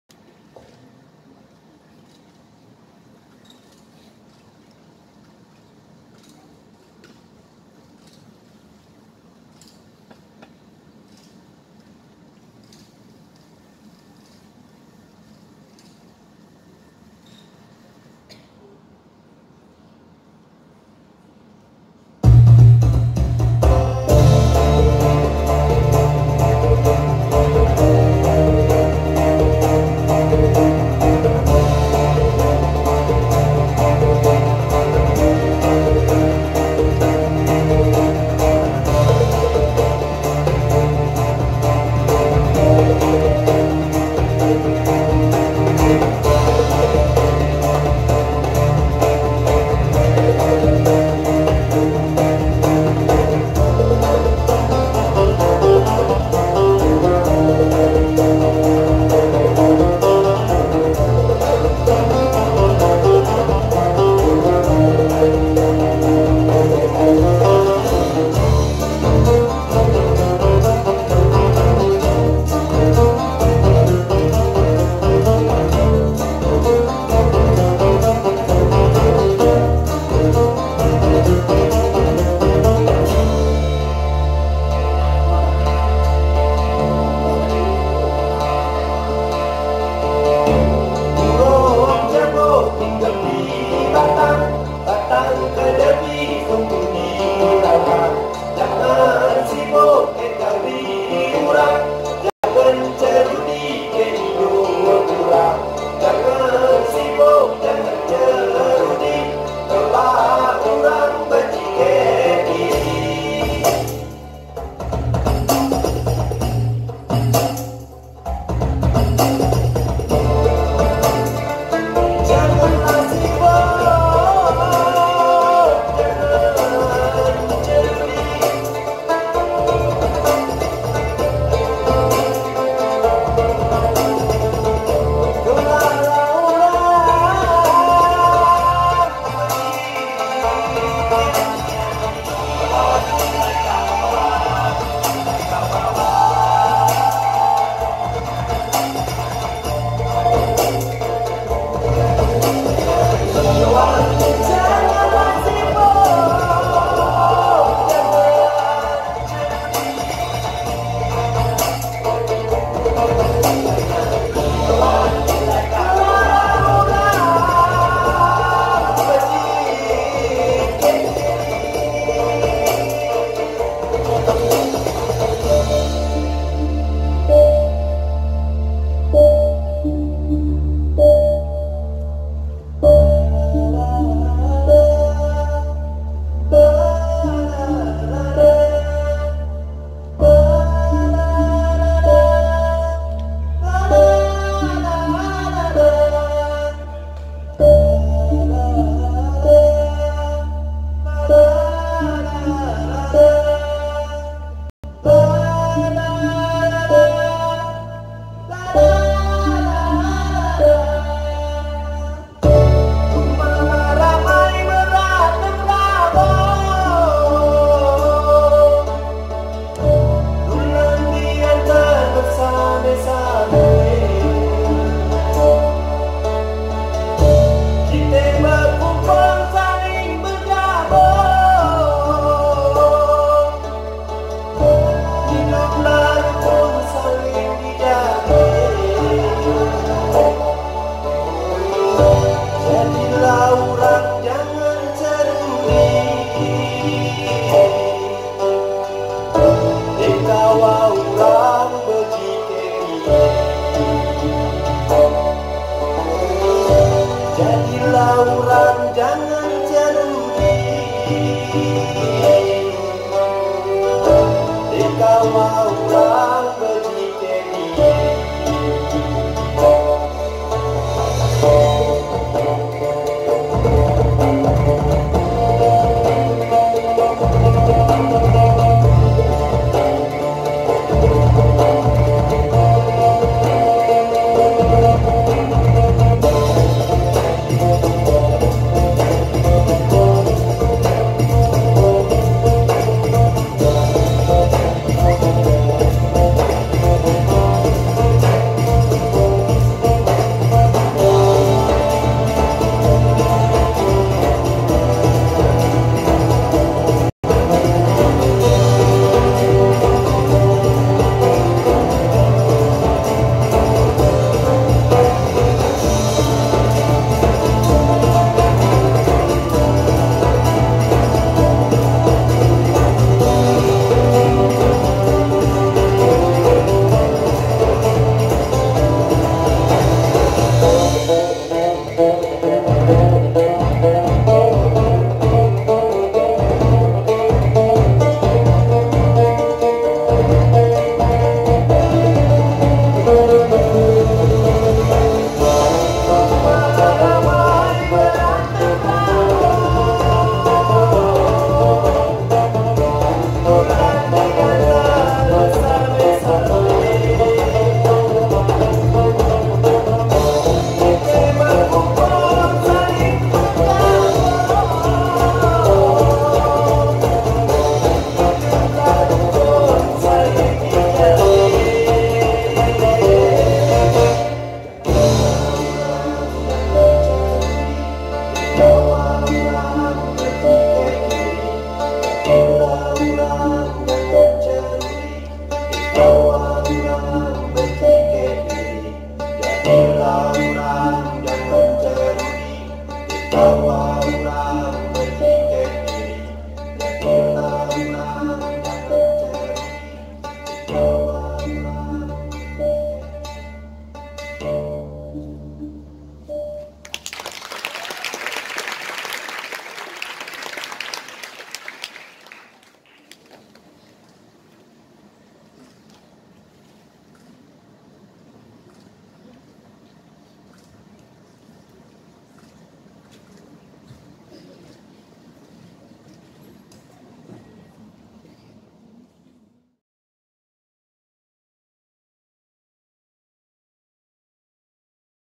Tari Cerudik. Koreografer Tasya Dwi Hartanti. Pemusik Dodi Kuniawan & Adishwara.
"Jadilah urang jangan cerudik dak kawa urang bejik kek dirik".

Garapan tari tradisi kreasi yang berpijak pada gerak campak ini menceritakan dampak lingkungan sekitar terhadap orang yang memiliki sifat suka membicarakan orang lain yang di sebut cerudik.